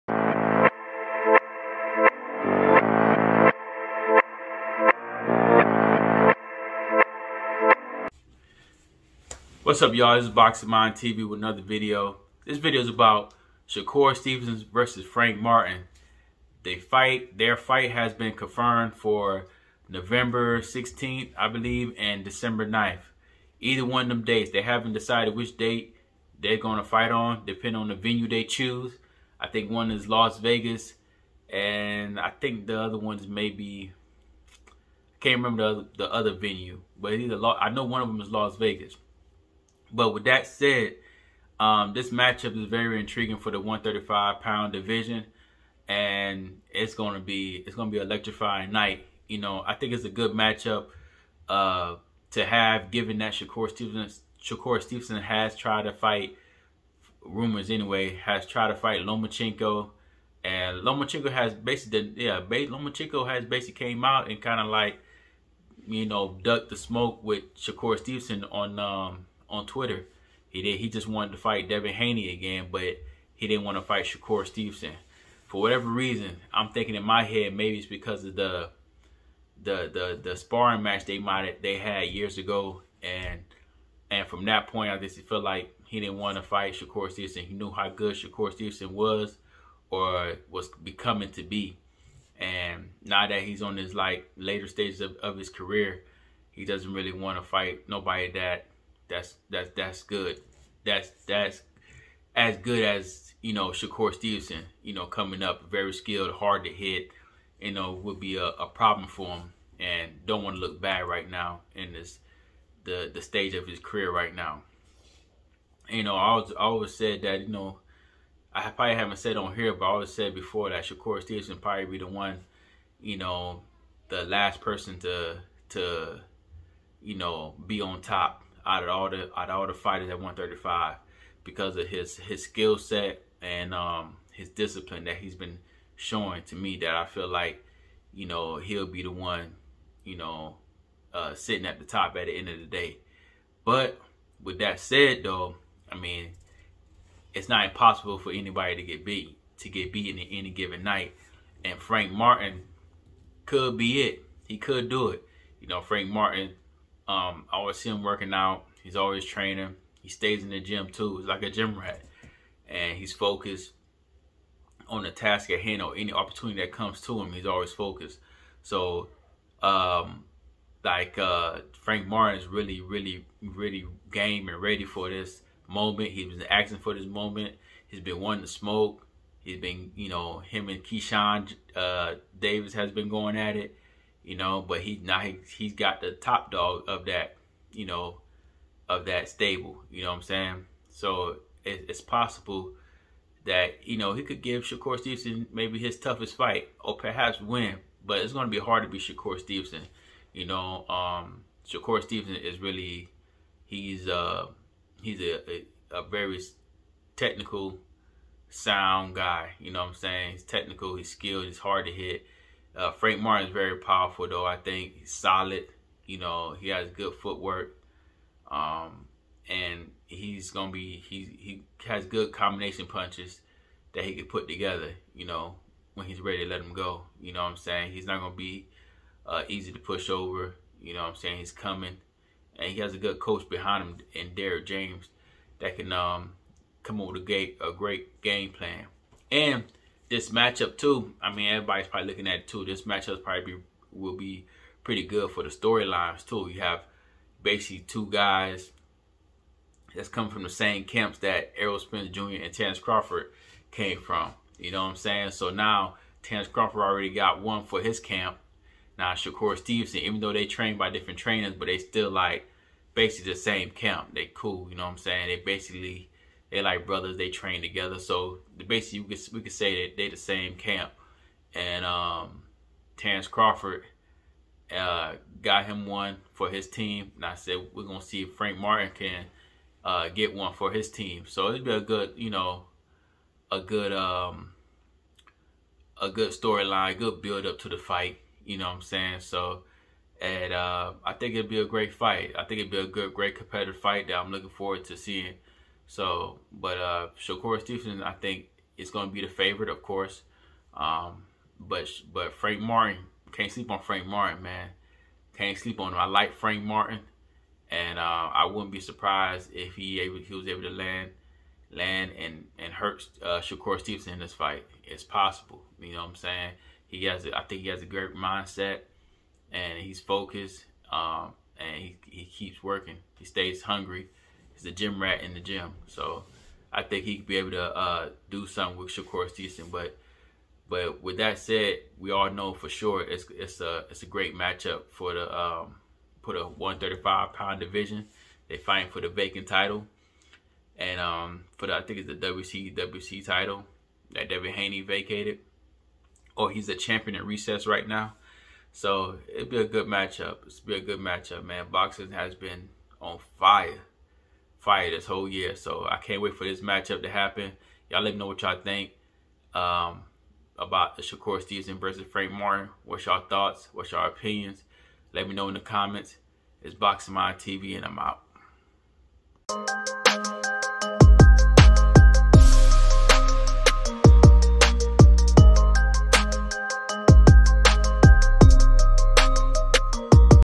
Shakur Stevenson vs Frank Martin Is ON!/ my thoughts
Shakur Stevenson vs Frank Martin is happening. What do you think the outcome will be?